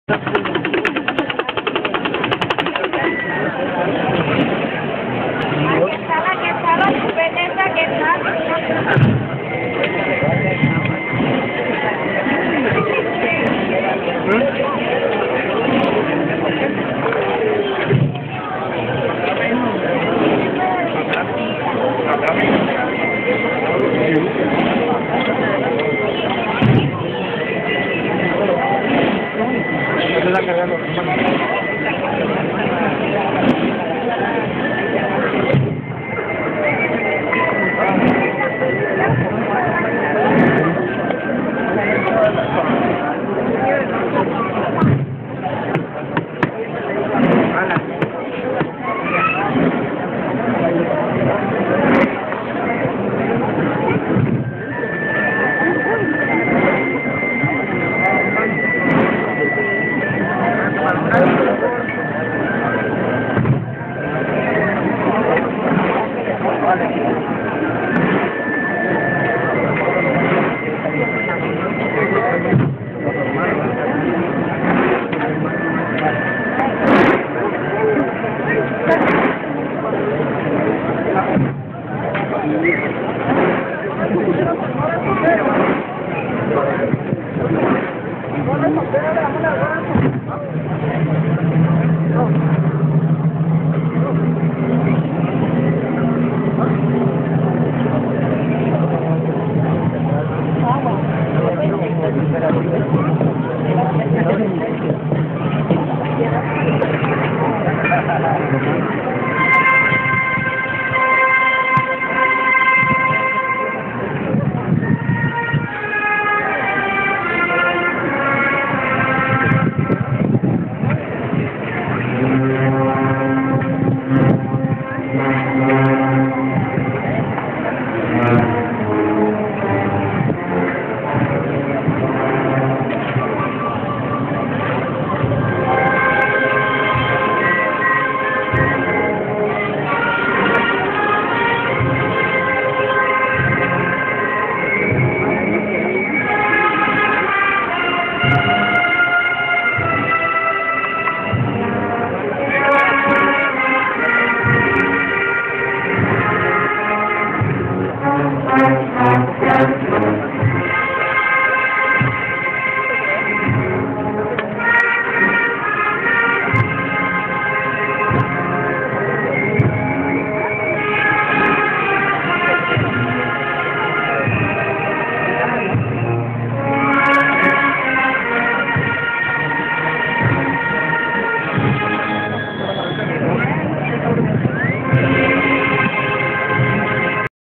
Procesión de Jesús de la Justicia por el Parque Colón - 2,013
Tradicional procesión de la Consagrada Imagen de Jesús de la Justicia, Templo de El Calvario. Segundo Domingo de Cuaresma 24 de Febrero 2013, Guatemala. C.A. Tradición y Devoción en Guatemala - Unidos por una misma fe...